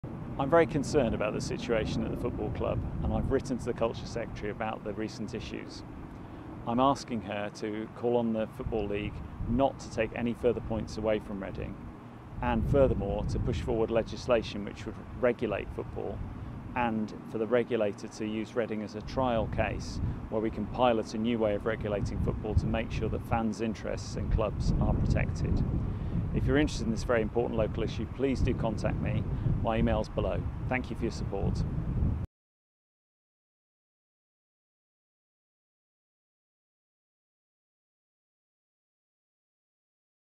Reading FC's situation: My requests to the Culture Secretary
I am very concerned about situation at Reading FC - Multiple points deductions which are largely due to the owner’s financing of the club, rather than the events on the pitch.

I have I have written to the Culture Secretary with four requests regarding Reading FC:

1. That the EFL make no further points deductions in this case.

2. Speed up the introduction of new legislation on regulating football, introducing a new football regulator.

3. Allow Reading FC to be part of a pilot of the new regulatory system.

4. See what more can be done to address the fans’ concerns about the club’s owner.